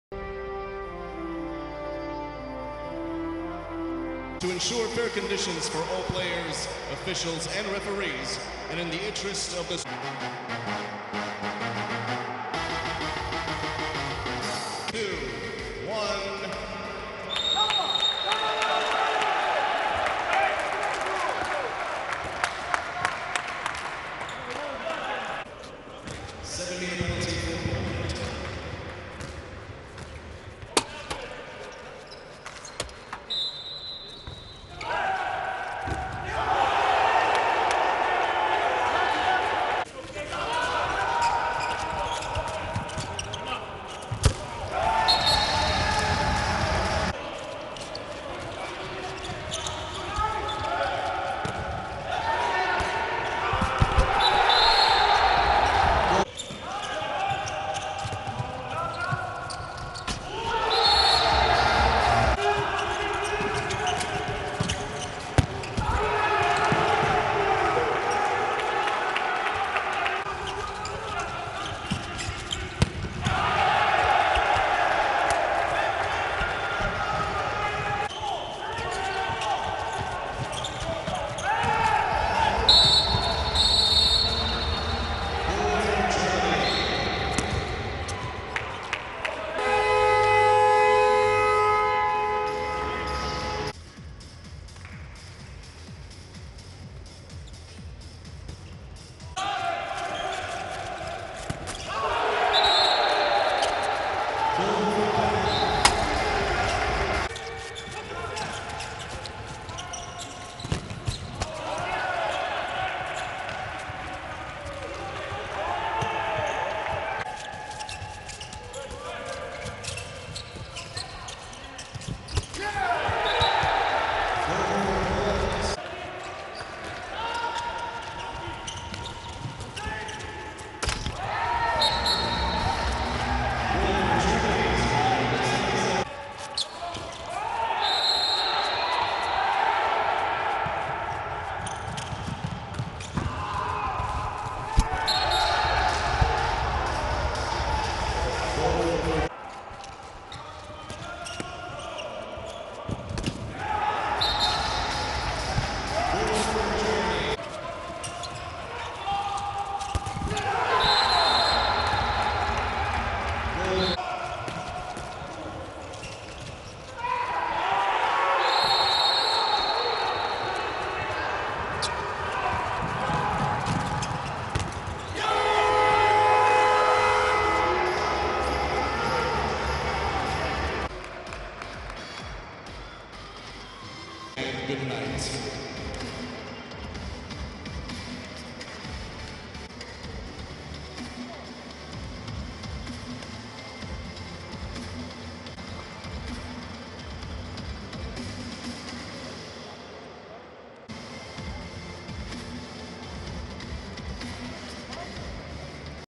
POLSKA - NIEMCY rok temu w Egipcie
@timeout.handball .W styczniu 2021 reprezentacja Polski rozegrała niezły mecz z Niemcami zakończony remisem szczęsliwym dla Niemców. Czy po roku mamy prawo oczekiwać lepszej drużyny i zwycięstwa ? Jestem umiarkowanym optymistą. Morale zwycięzców na EURO 2022 może zbudować tylko wygrana w pierwszym meczu z Austrią , który będzie kluczowym dla rozwoju naszej drużyny. Przypomnijmy sobie jak reprezentowała się nasza reprezentacja rok temu .